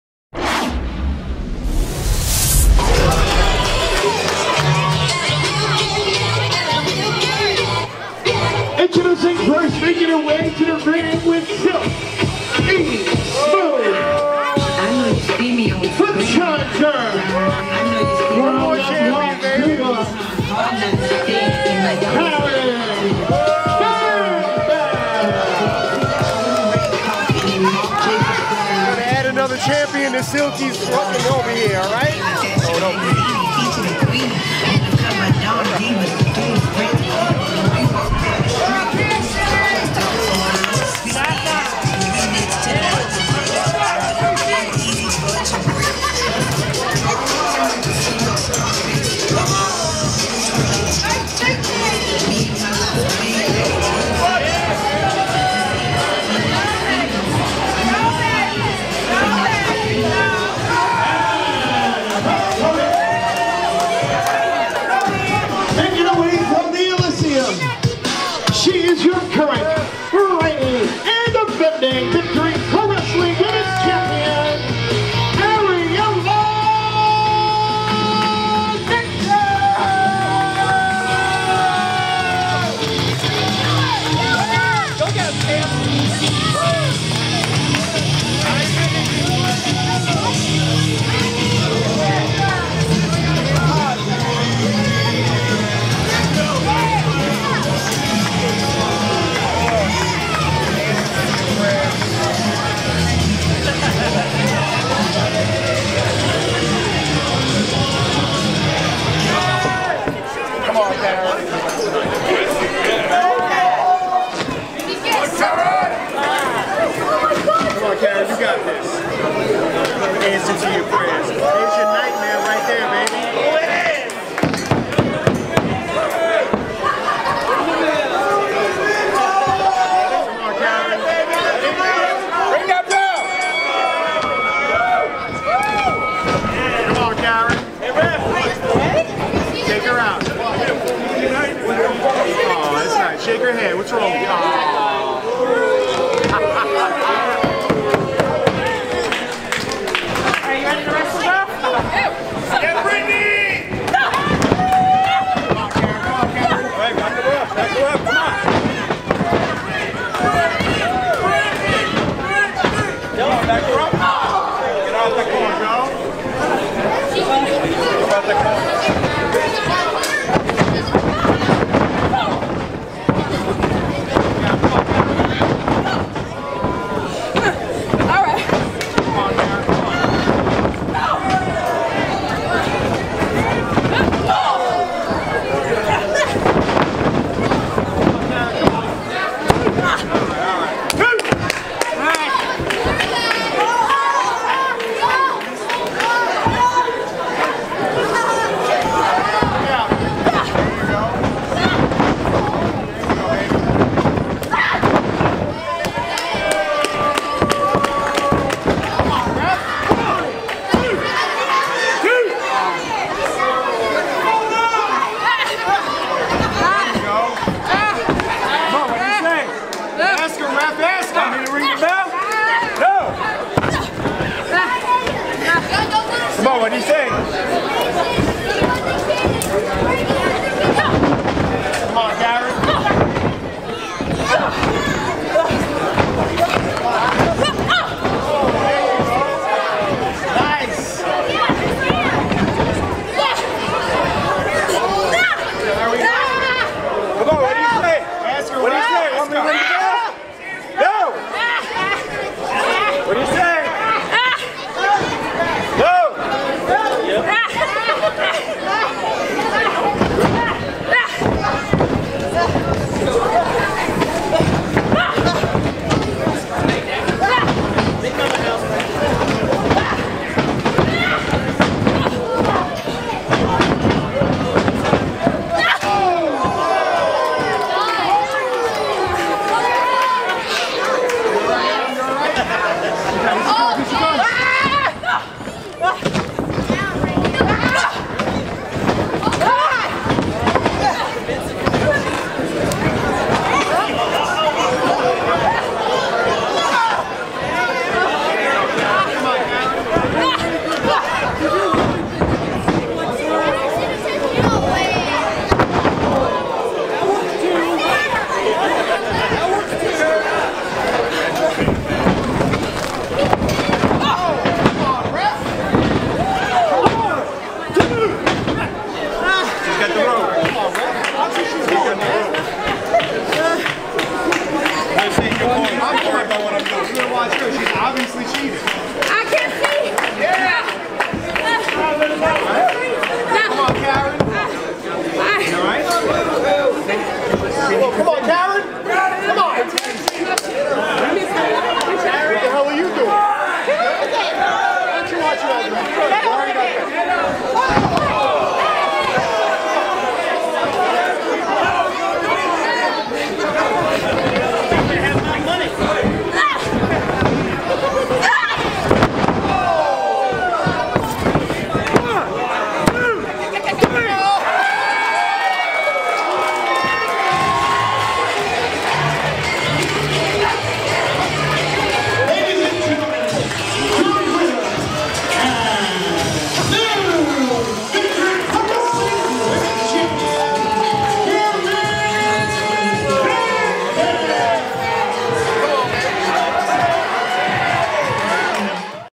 VPW Women's Championship - Karen BamBam vs Ariela Nyx
From Victory Pro Wrestling in Centerreach, New York.

King of New York 2020